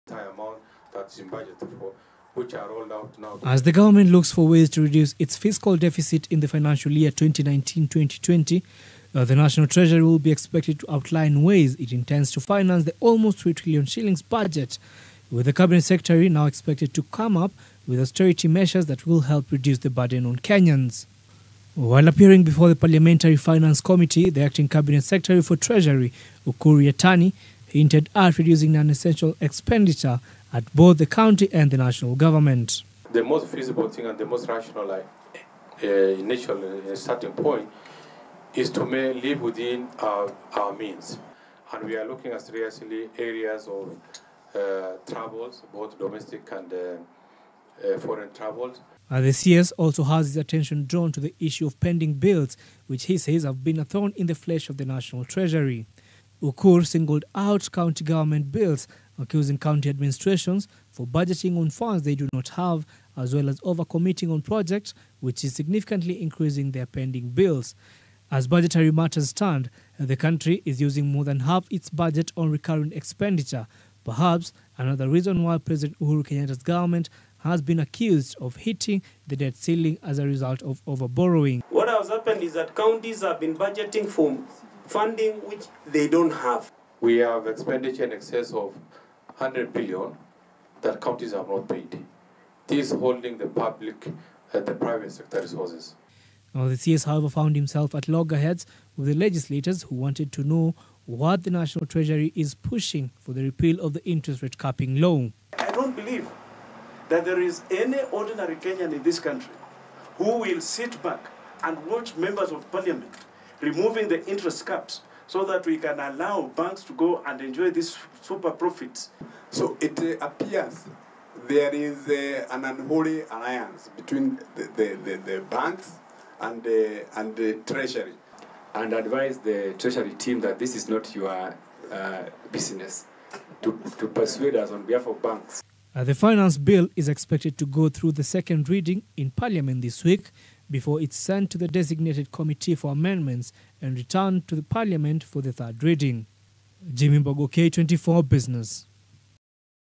Finance Bill 2019 top on agenda as parliament resumes
As parliament reconvenes tomorrow among key issues set to be given priority is the finance bill 2019, in which the national treasury has made proposals that they hope will help them finance the 2019, 2020 budget.
Among the proposals in the bill is the repeal of section 33b of the banking act to remove the interest rate capping law which the national treasury cabinet secretary says has not achieved its intended purpose.
The bill also seeks to reduce government expenditure while at the same time increasing revenue collection through the Kenya revenue authority.